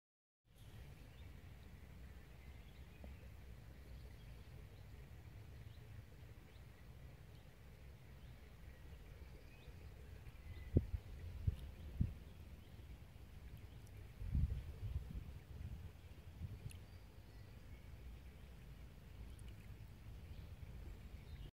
Loch Lomond Nature Reserve
Loch Lomond Nature Reserve - A beautiful day in the west of Scotland and an amazing view